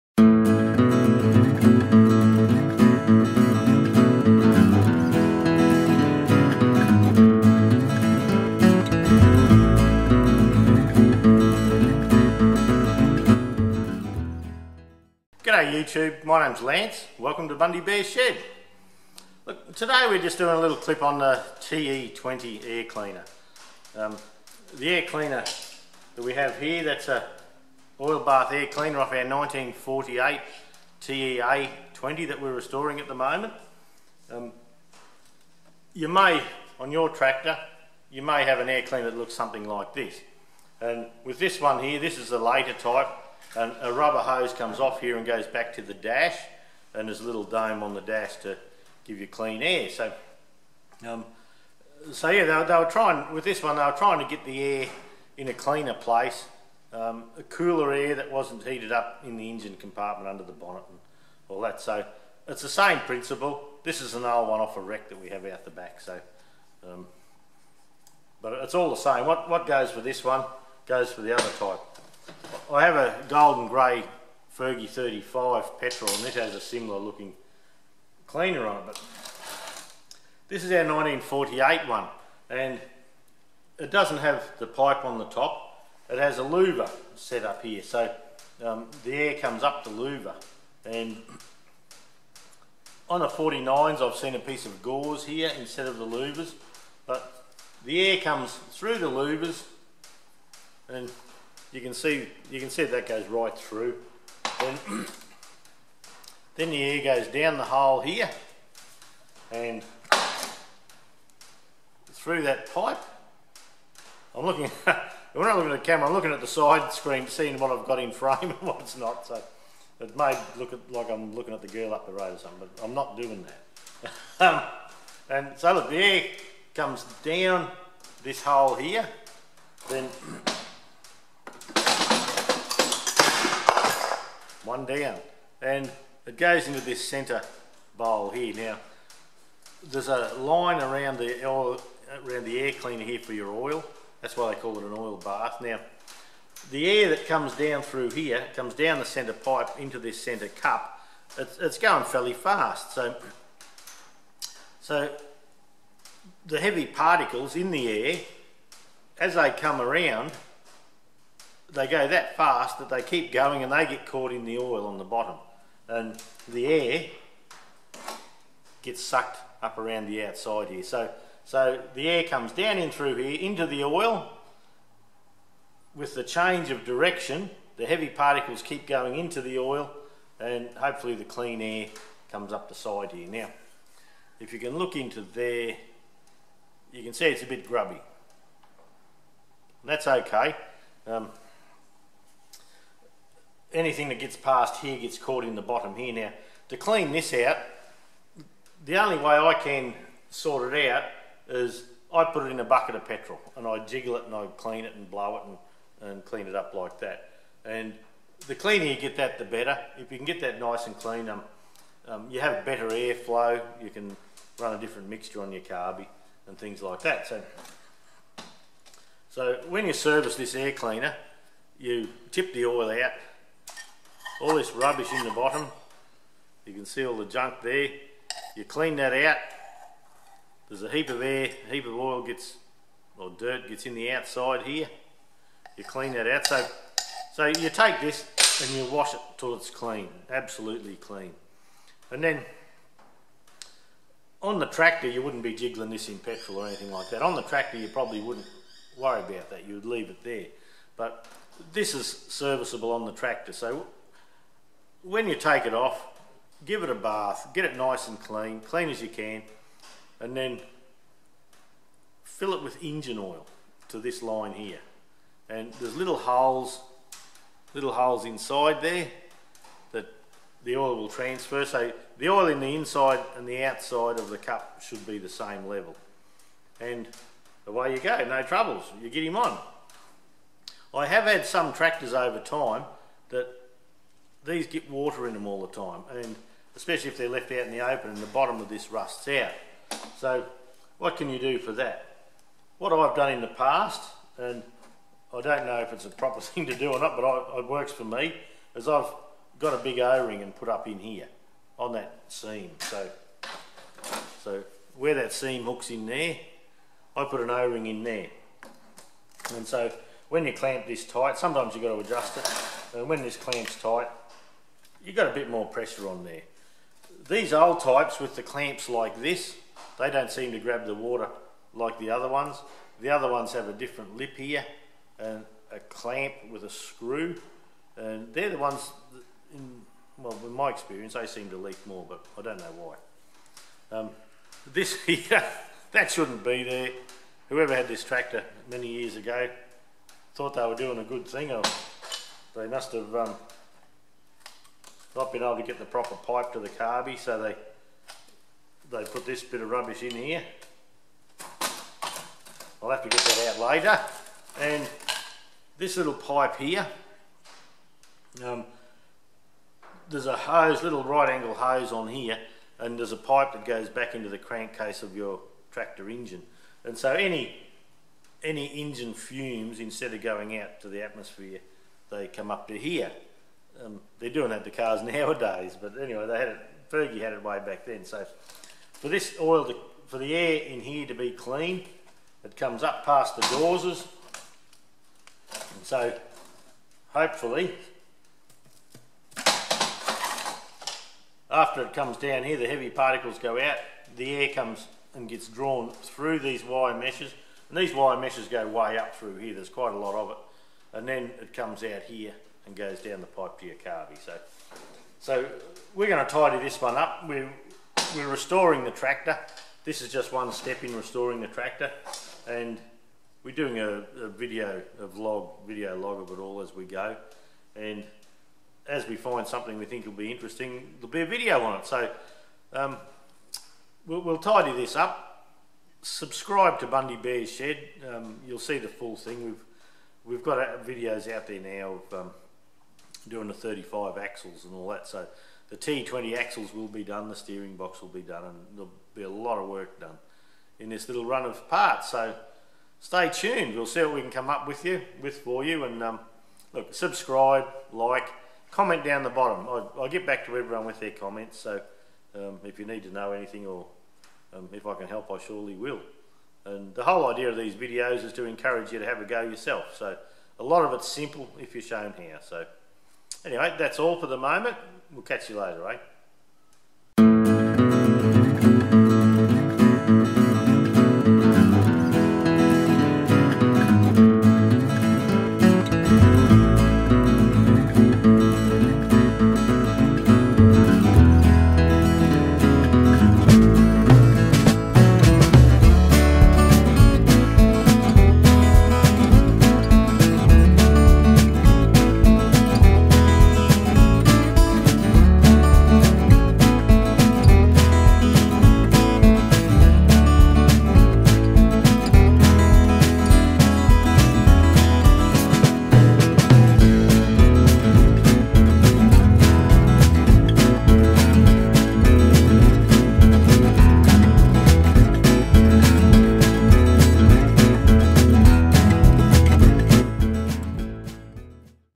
How does your Ferguson TE20 Air Cleaner work?
In this video I have a chat about how the oil bath air cleaner works and what to service.